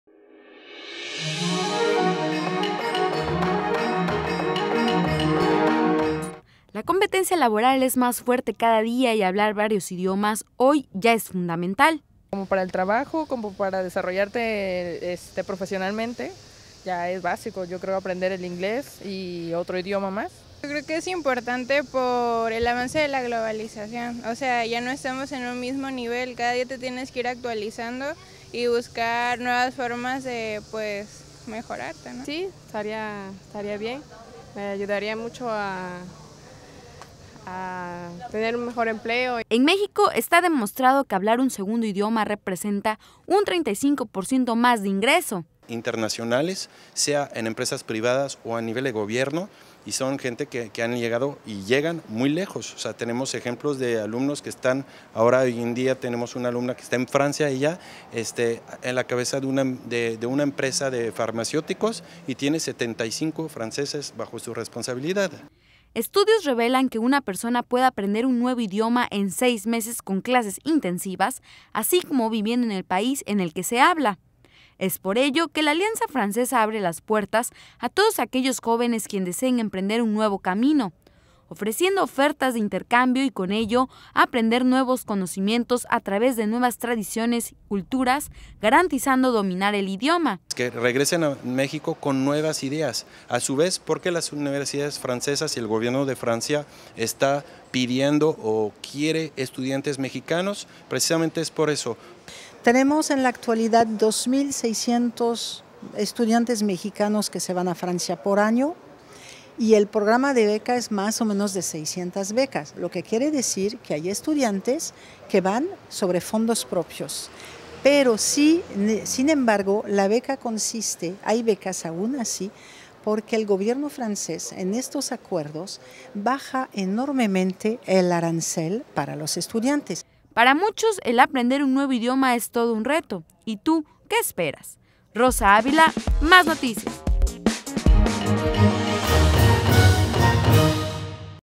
Jóvenes buscan aprender otros idiomas
Hoy en día, los jóvenes buscan mejorar su calidad de vida, es por ello que optan por aprender otro idioma.